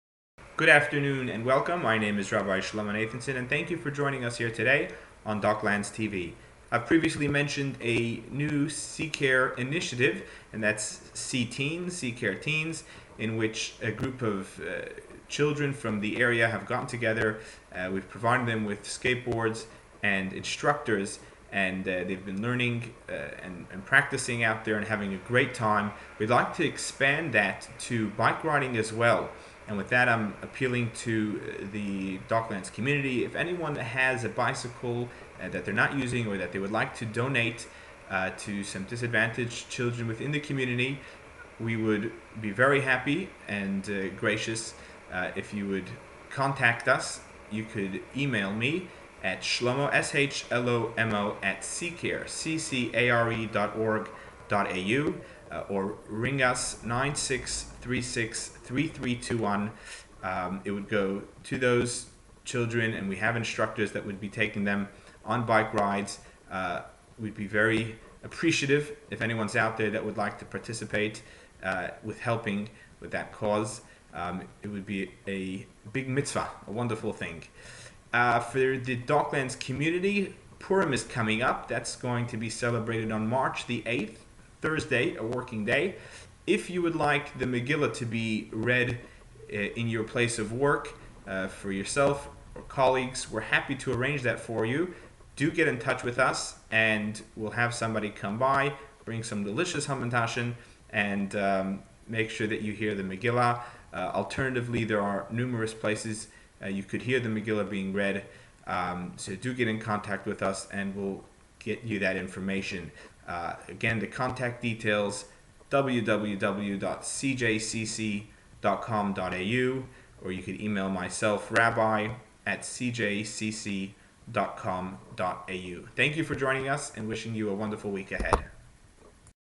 February 20, 2012 - Jewish Community Centre - Docklands TV
Shlomo speaks about "C Care Teens" and Bike group appeal. Info for the upcoming holiday of Purim.